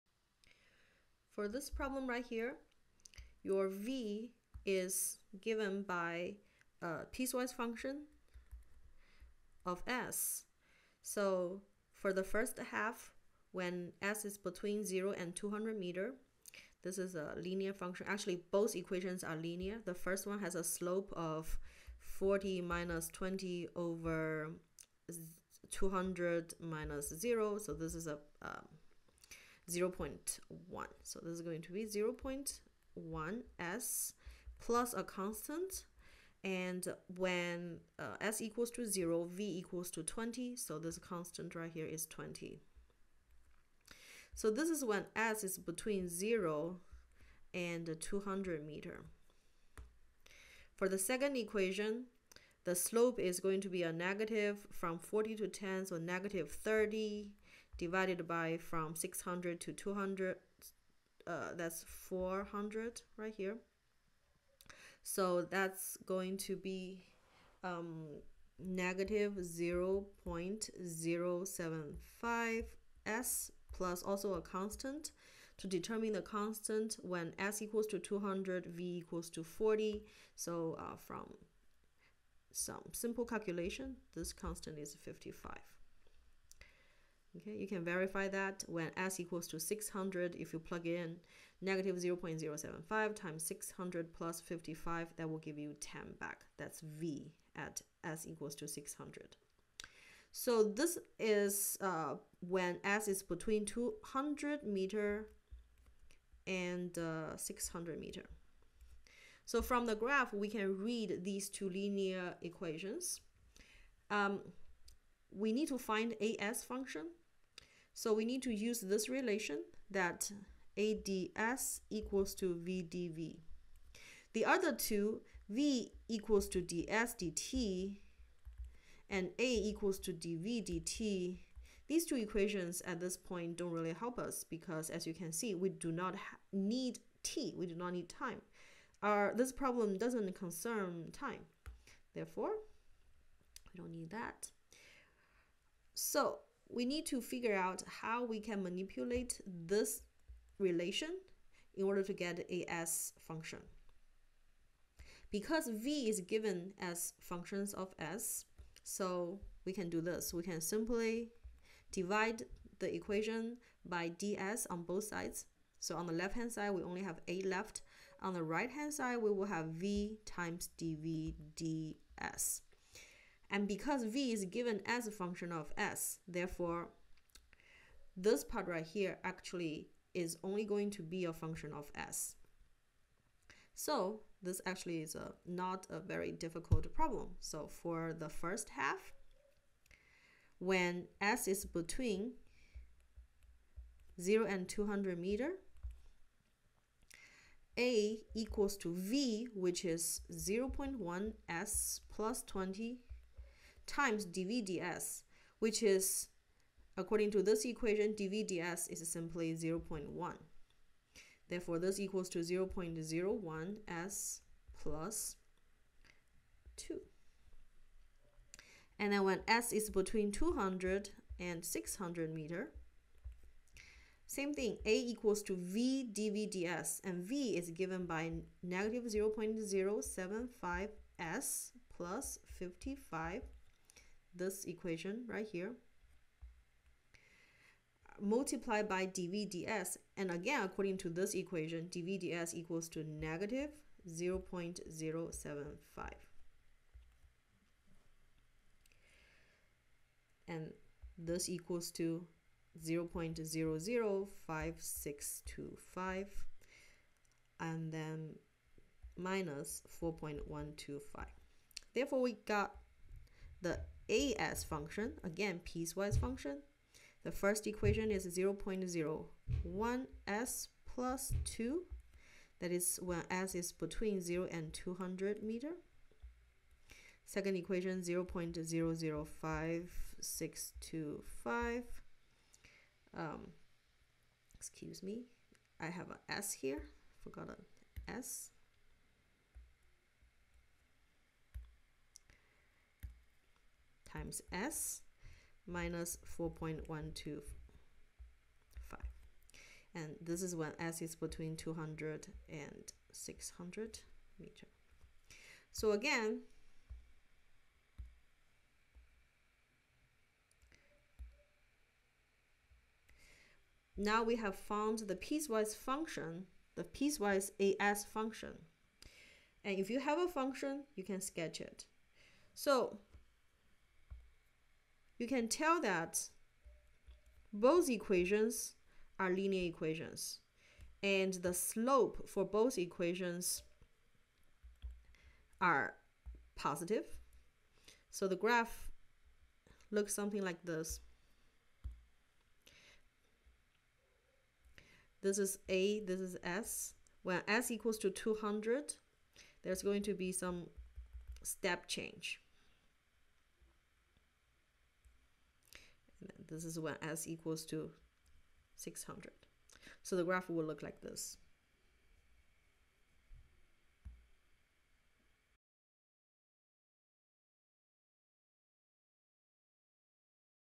Dynamics W HW01P6 D05Q4 9-8-2016
Video solution of Dynamics written HW 01, problem 5, which is also the quiz problem 4 in video